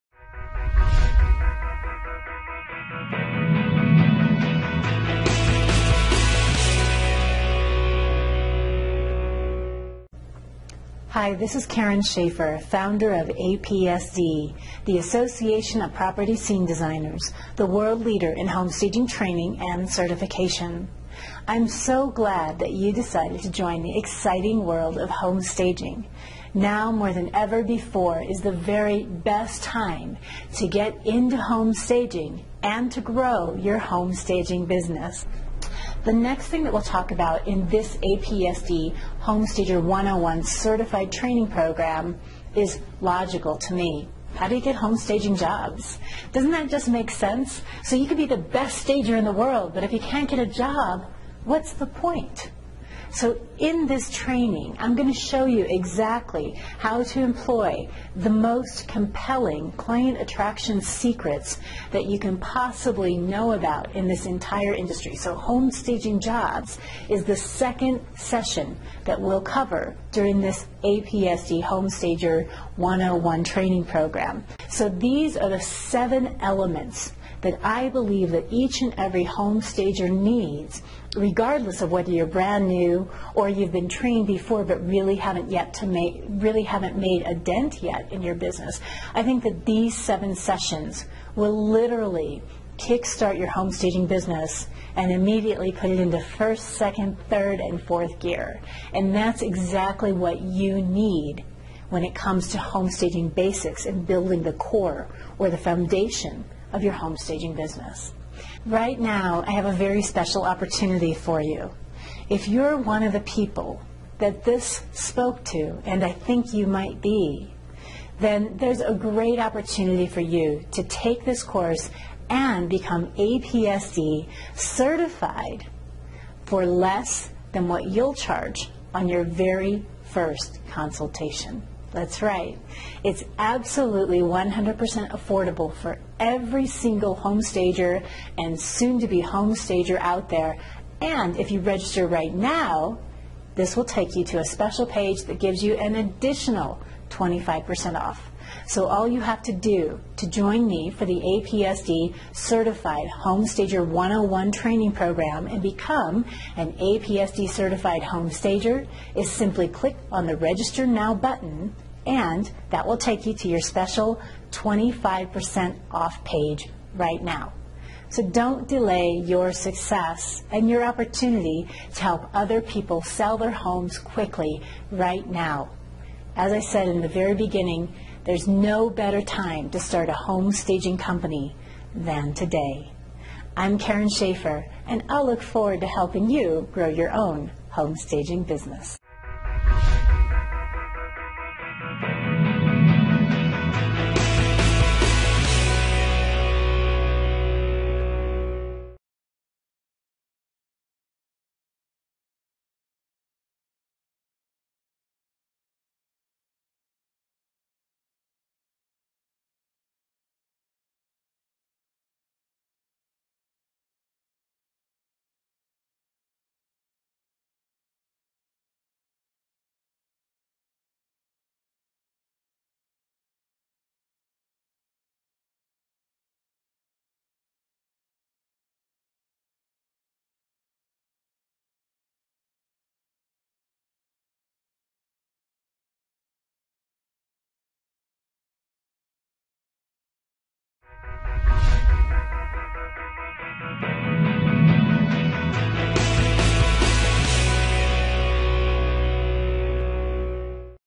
How to Get Home Staging Jobs - Part 2 of 7 APSD Home Staging Business Buliding Series
How to get Home Staging Jobs. In this video, I will introduce you to:
How to find sellers, attracting the right agent and working with investors
You can be the best stager in the world but unless you know how to attract clients and confirm jobs then it doesn't really matter. This section tells you exactly who you should be marketing to, how to attract them and then how to secure the home staging job. | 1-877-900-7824